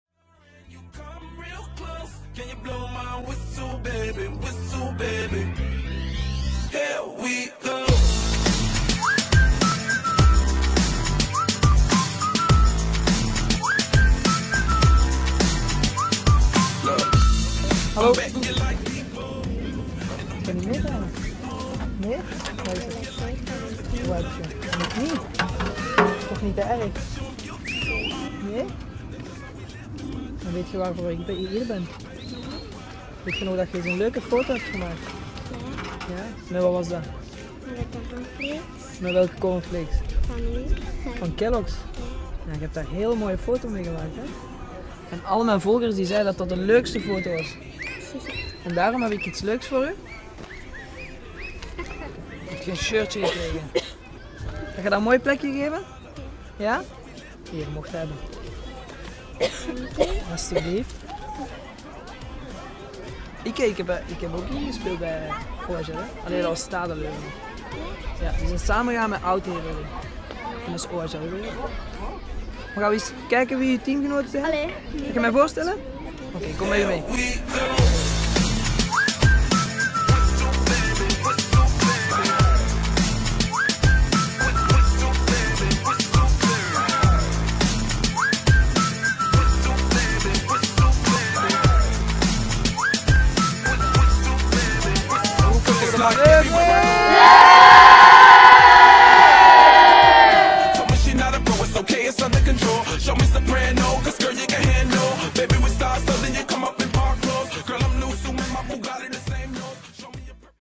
Op bezoek bij Xoentho Janssen (OHL), de winnaar van de Kellogg's-actie.
Om de winnaar van de Kellogg's actie zijn truitje te bezorgen mocht ik op bezoek bij mijn oude club: OH Leuven. Xoentho Janssen stuurde volgens jullie de mooiste foto op. Ik hoop dat hij er blij mee is!